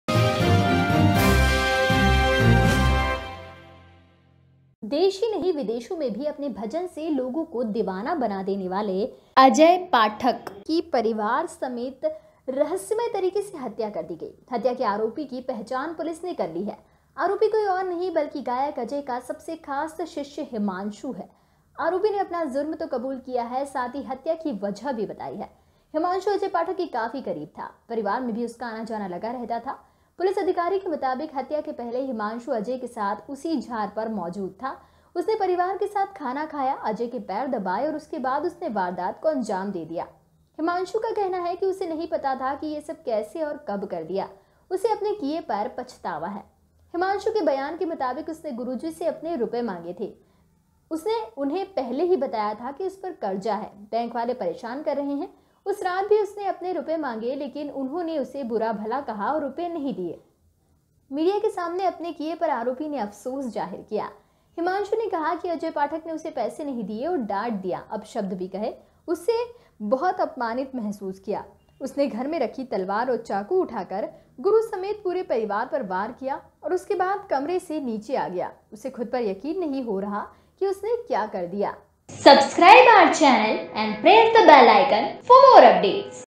गुरुजी के चहेते इस शिष्य ने गुरु के परिवार का सफाया क्यों किया
पुलिस के सामने भोला बनता रहा फिर हुआ पर्दाफाश | Ghamasan News is India's Biggest Hindi news Portal.